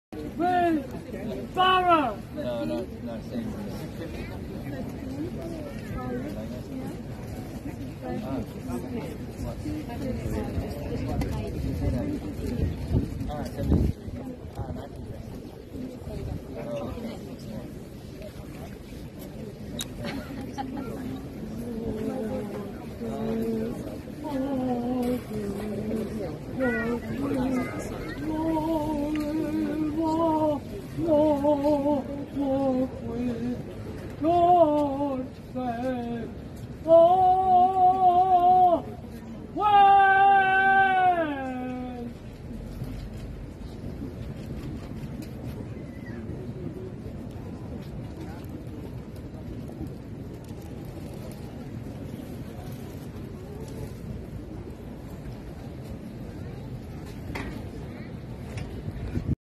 Множество пред Бъкингамския дворец, за да отдаде почит на кралицата
Автор на видеото: Анастасия Страволемова